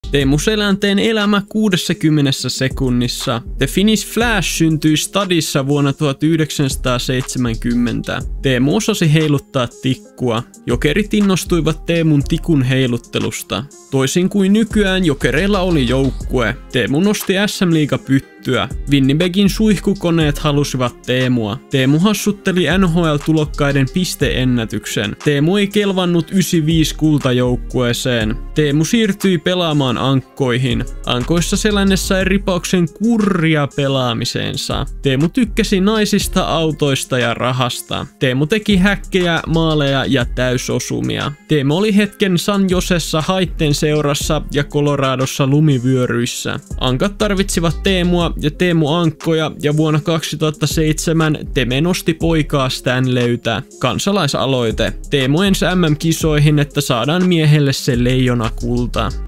Teemu Selänteen elämä 60 sekunnissa
Jääkiekkoilija Teukan elämä ja teot

Uusia videoita tulossa joka viikko!